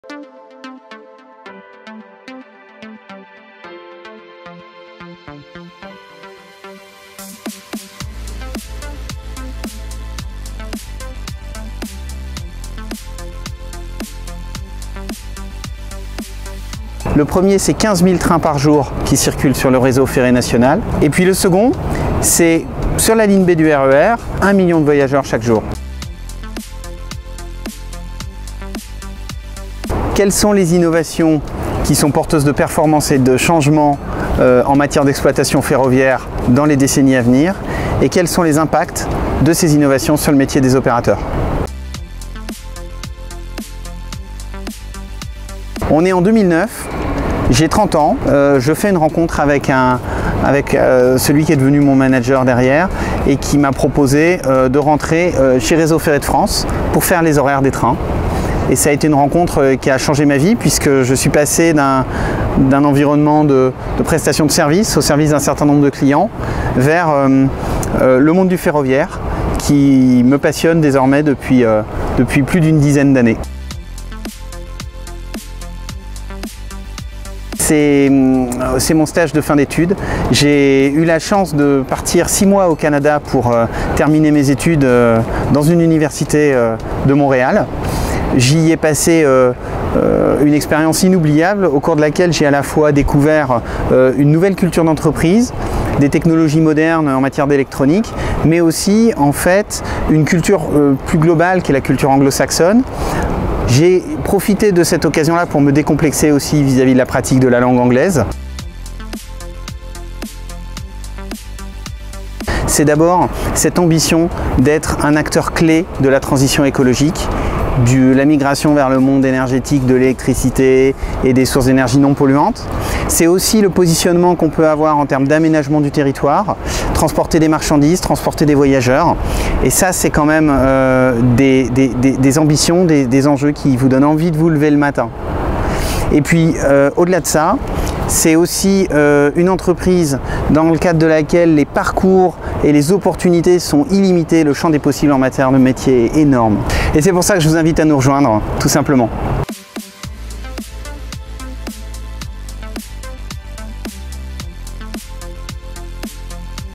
Pierre Boutin - SNCF Réseau : "Le champ des possibles en matière de métiers est énorme"
SNCF en quelques chiffres-clés ? La question à poser sur son job ? Sa plus belle rencontre professionnelle ?... Pierre Boutin, Directeur de la transformation et des compétences d’exploitation de SNCF Réseau, répond à nos 5 questions en moins de 5 minutes. Interview vidéo GiveMeFive.